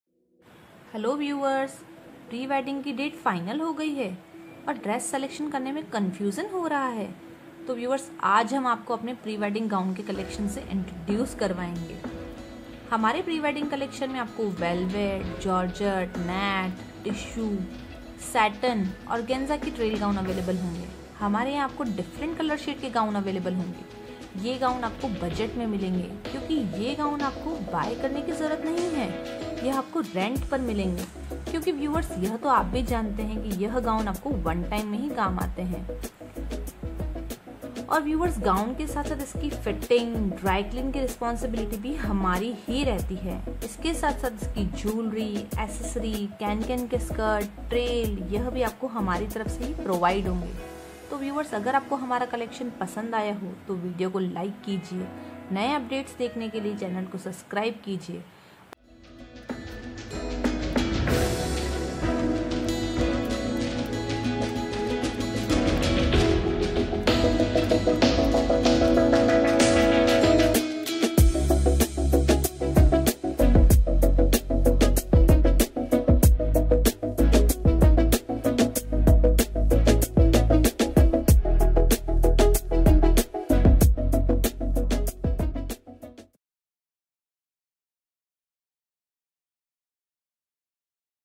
Top 25 Prewedding Gown on Rent
In this video We are talking about Pre-Wedding Gowns which is available on rent in Udaipur City. The trend of Pre-Wedding gowns has increased a lot these days. Nowadays all the couples are getting Pre-Wedding done, for which a lot of Pre-Wedding Outfits are needed, Keeping this need in mind, we have brought a Pre-Wedding Collection for you, that too on rent, so that you will not have to spend much money and your work will be done in less money.
In our Services, You are given all the gown as well as its fitting cancan skirt, Trail & Jewelry.
Plain Gown, Tissue Gown, Organza Gown, Velvet Gown, Georgette Gown are available on different color option.